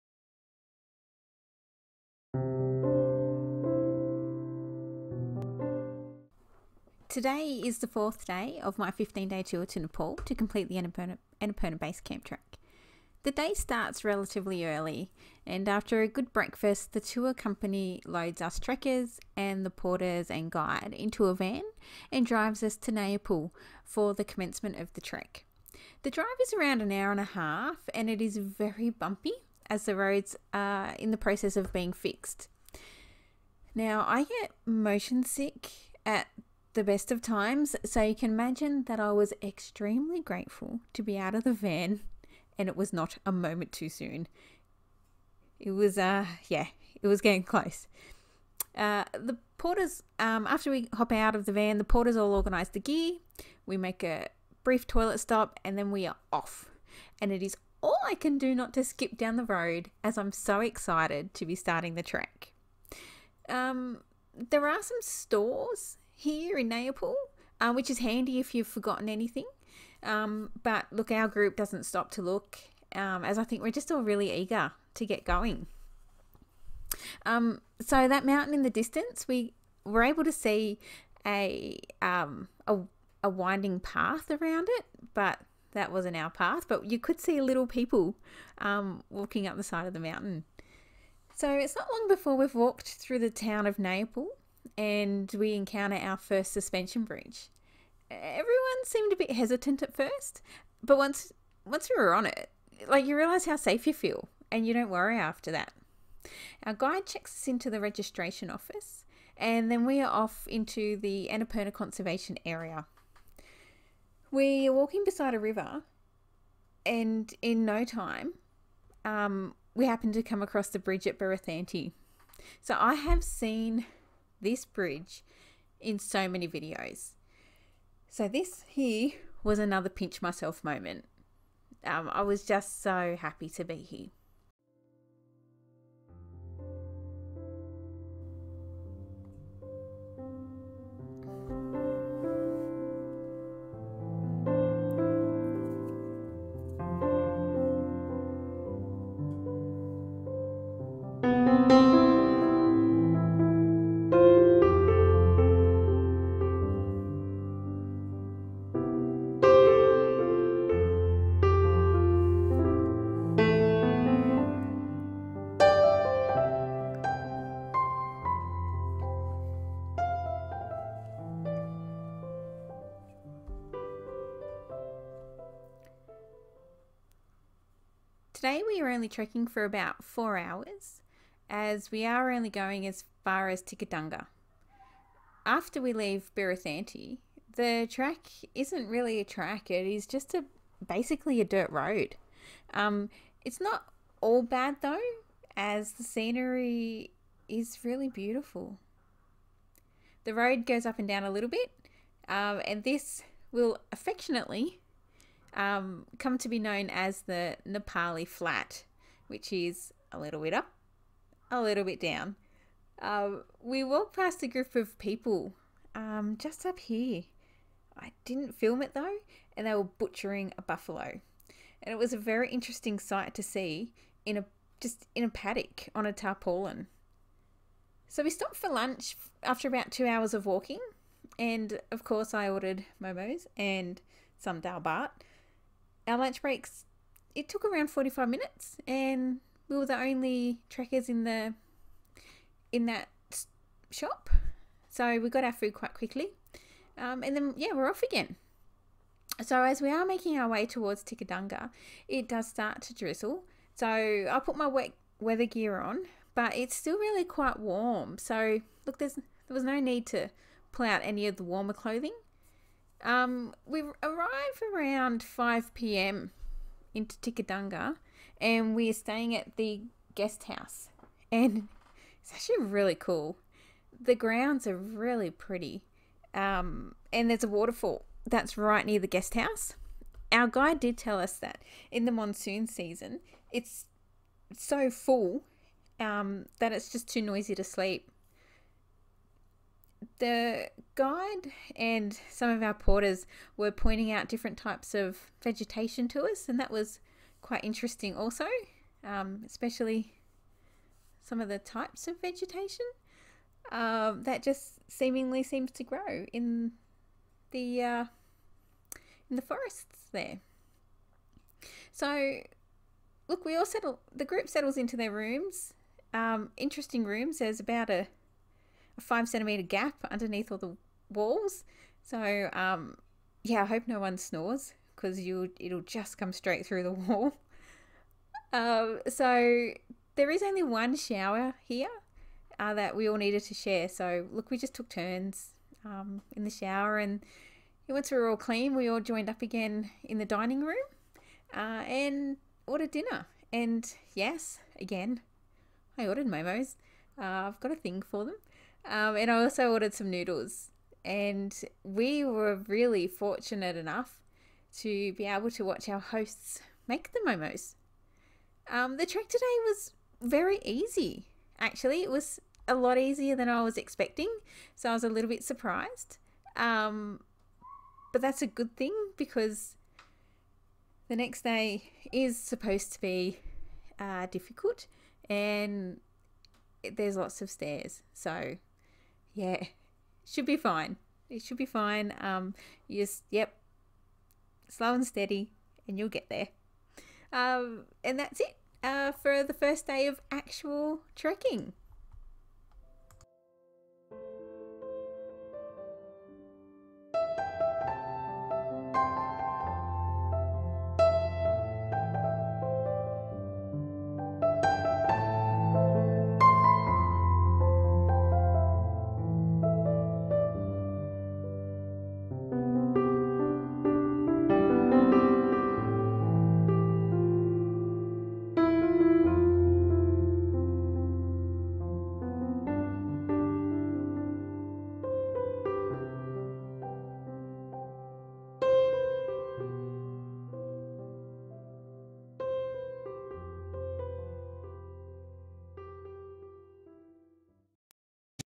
Pokhara - Nayapul - Tikhedhunga || Annapurna Base Camp Trek || Day 4
Day 4 of my 15-day tour and trek to Annapurna Base Camp - 19 March 2019.

Today is the day I start trekking!!!

There is a ride in a van from Pokhara to the trailhead at Nayapul. It is a bit of a tummy churner and I'm surprised I didn't get carsick.

Not long after the group leaves Nayapul, we have to cross the first suspension bridge. I've been looking forward to crossing these.

And then I finally get to cross the iron bridge at Birethanti. I have seen it in so many other videos and I need to take a moment to compose myself as I can't quite believe I am here. It's amazing.

The trek today is mostly along a dirt road. It's not very difficult. I wish I knew to enjoy this day while it lasted because the next 8 days are going to be tough!